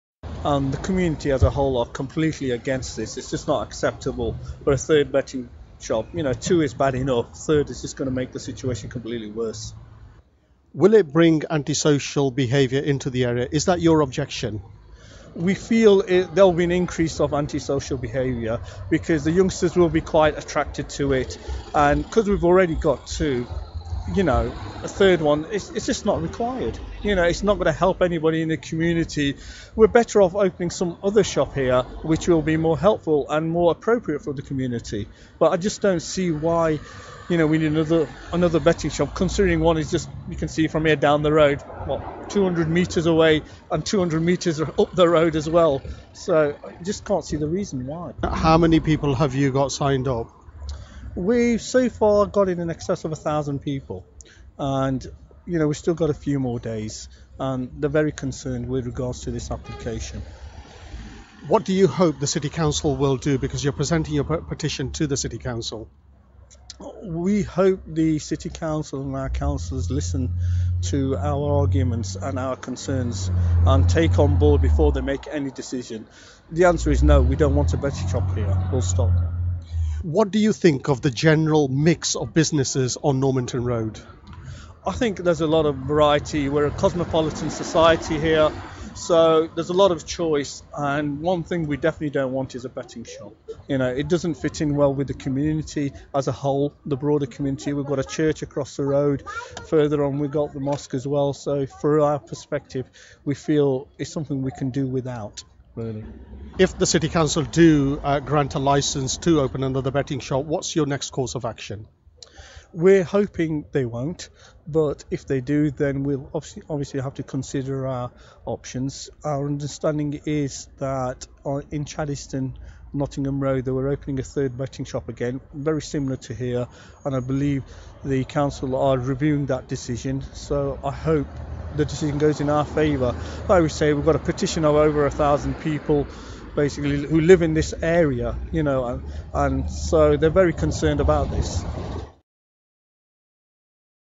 Derby residents petition against betting shop
Catch up with a feature on BBC Radio Derby about the people of Normanton Road in Derby, who have organised a petition to stop another betting shop opening in the area. There are already two betting shops on the road, and the organisers of the petition believe gambling encourages and attracts anti-social behaviour and takes money out of the community.